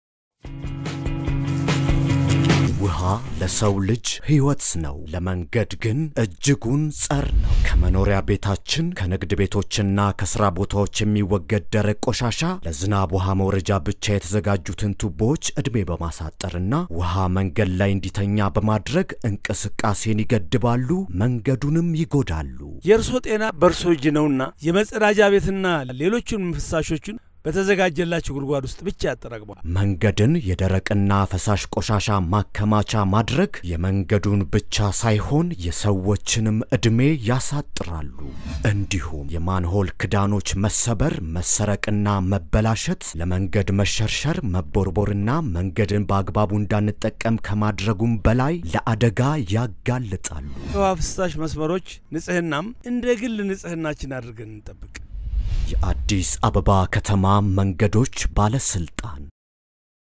drainage
የድሬኔጅ መስመሮች በቆሻሻ መደፈን መንገድ በውሃ እንዲጥለቀለቅ በማድረግ እንቅስቀሴን ያውካል፣ መንገዱንም ለብልሽት ይዳርጋል